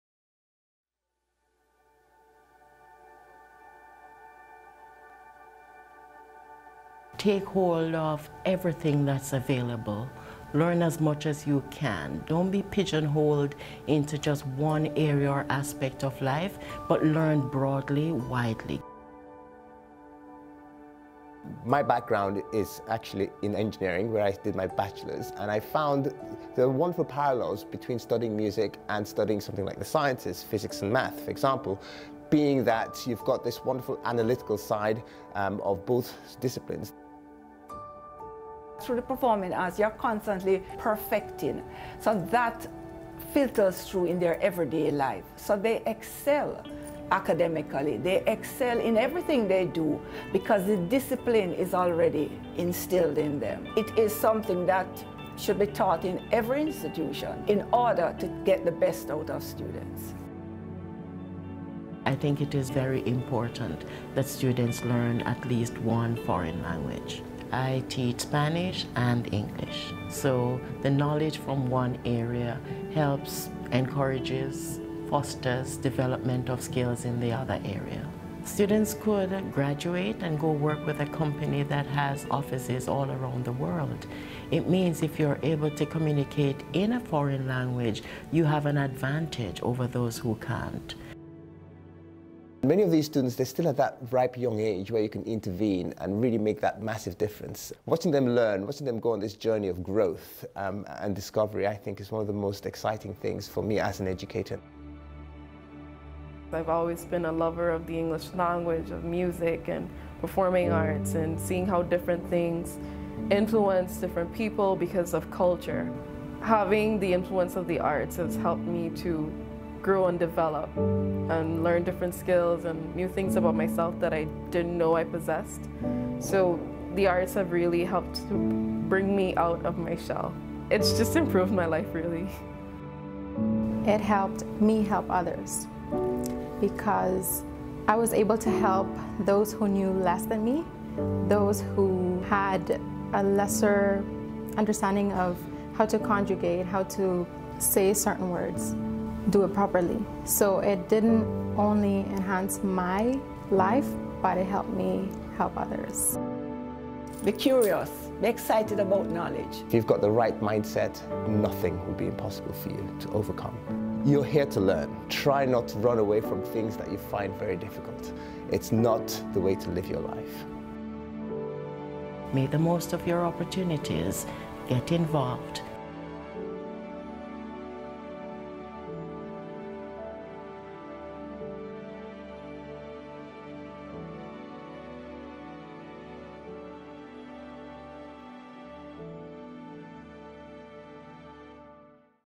We Are UCCI (S01E04) The Arts & Humanities at UCCI
An introduction to the Department of Arts & Humanities at the University College of the Cayman Islands